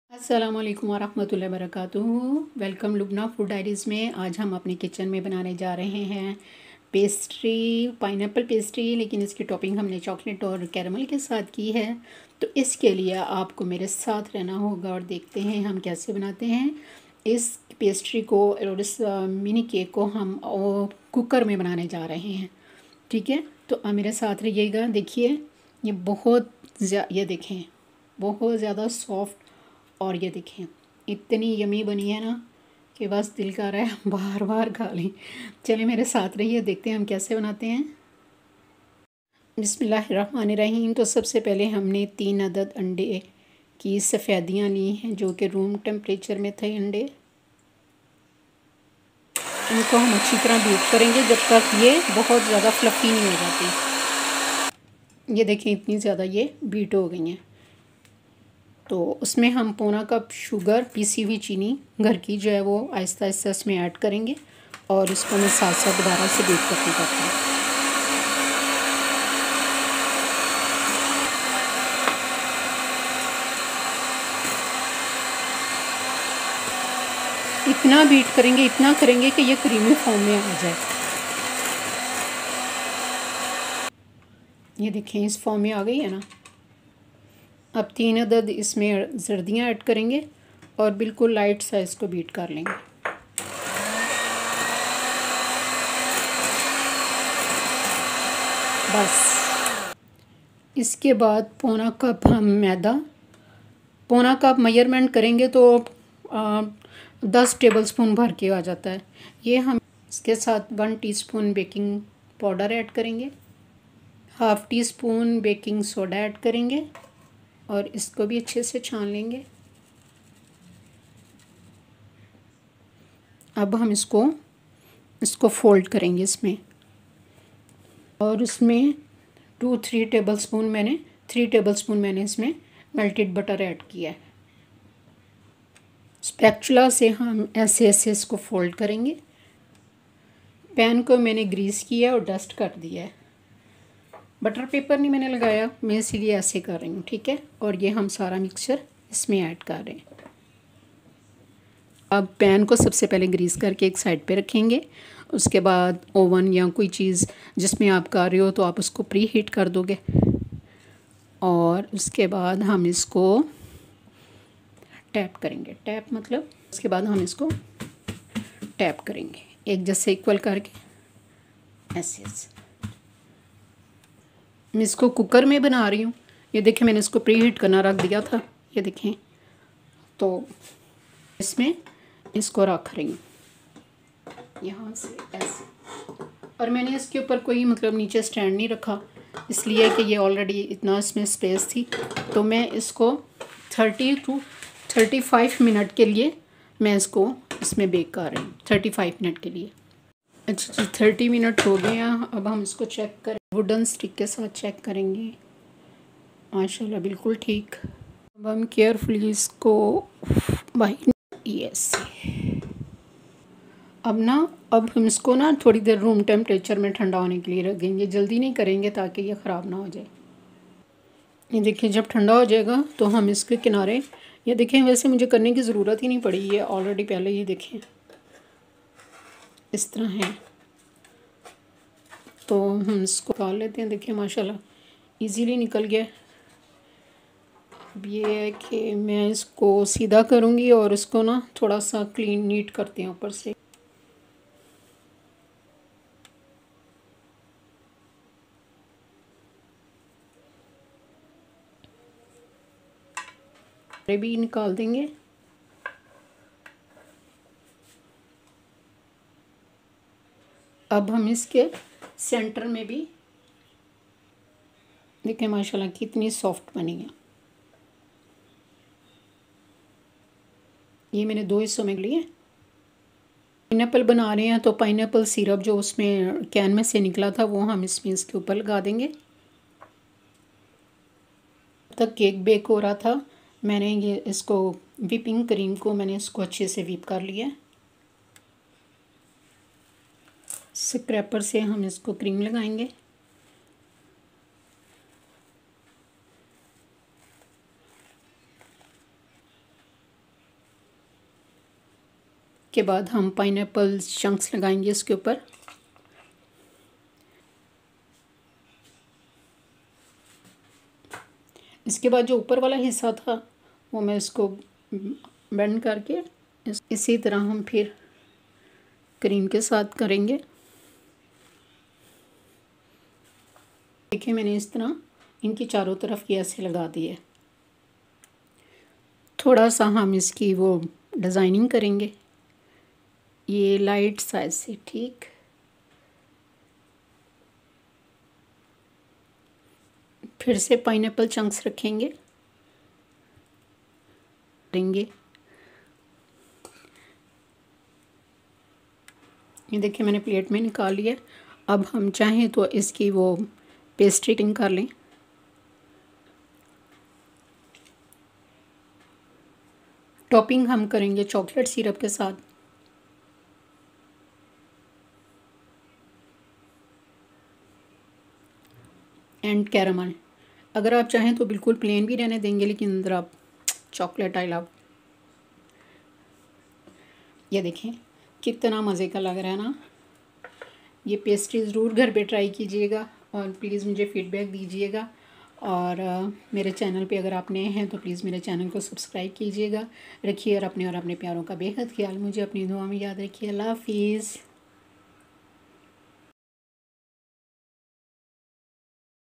pineapple pastry cake without ovenپاٸن ایپل پیسٹری بغیراوون کے
i m Lubna nd im house wife .cooking is my pashion
u learn how to make pineapple cake pastry at home
 i made cooker at home
its easy nd quick home made pastry
is realy delicious kids nd ealder love
these pastry
Lubna Food Diaries
is free of cost with bell icon for new update